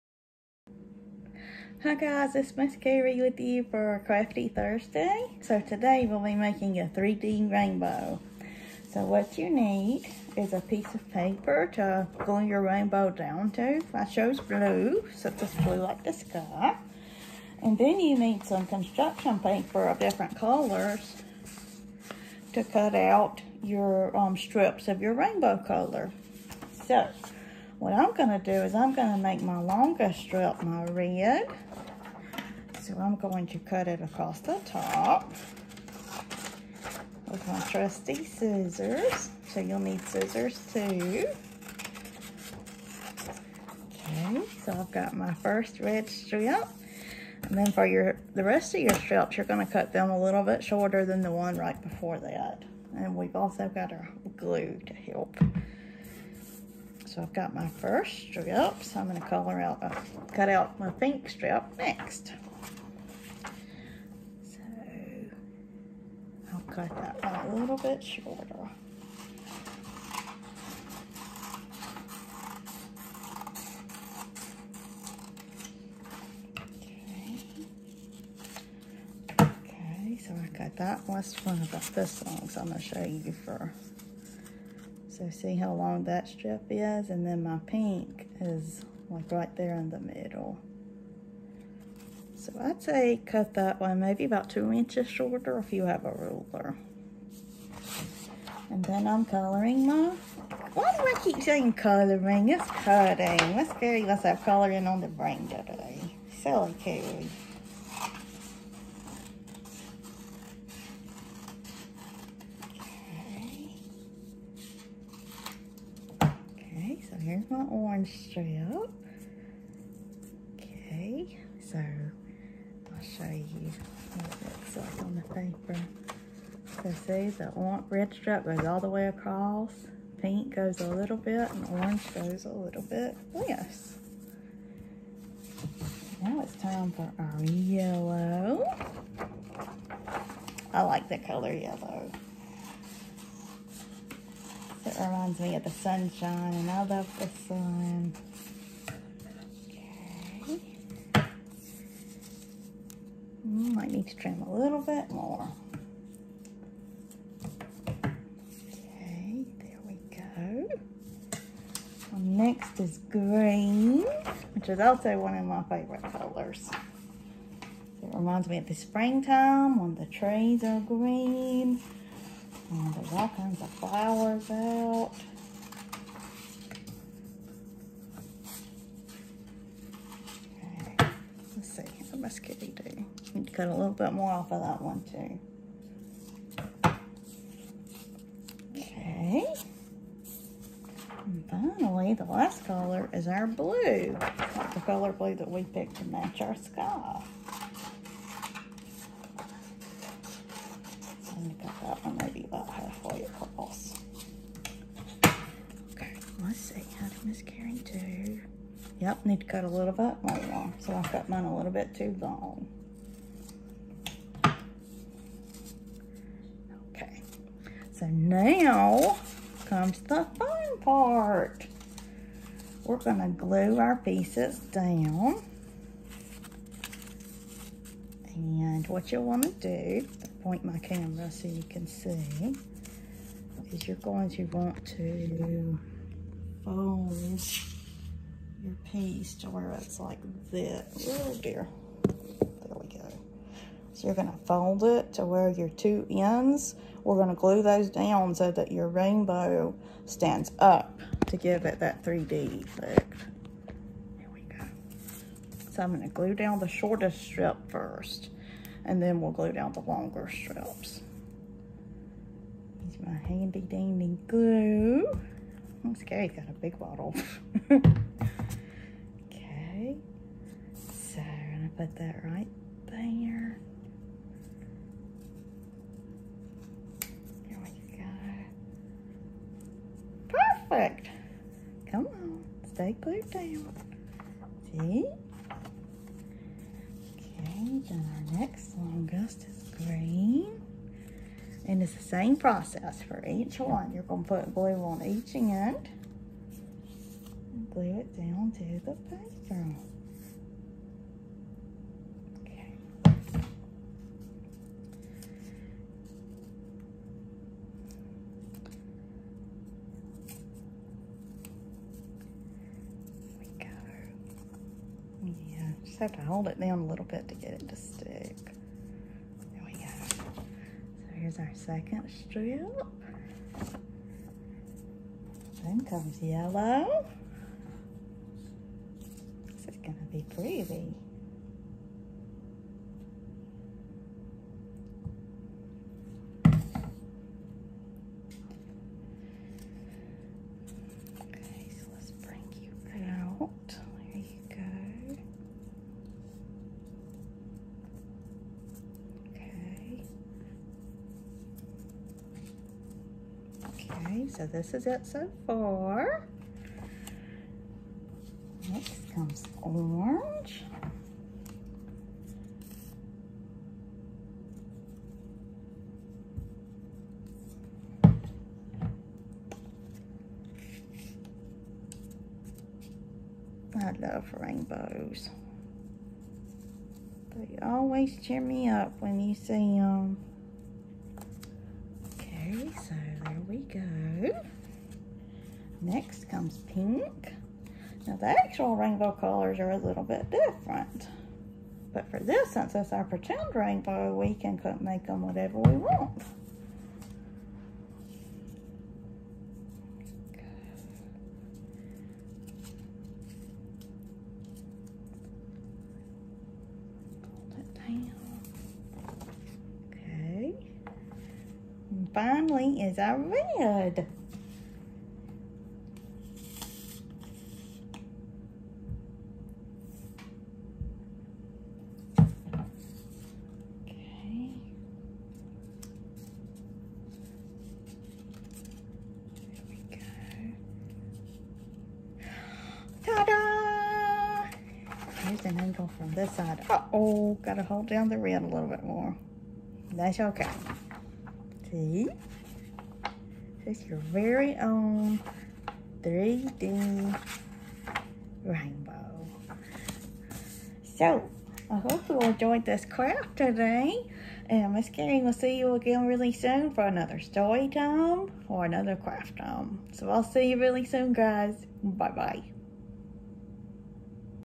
Crafty Thursday with Ms. Carrie
Today we're making 3D rainbows!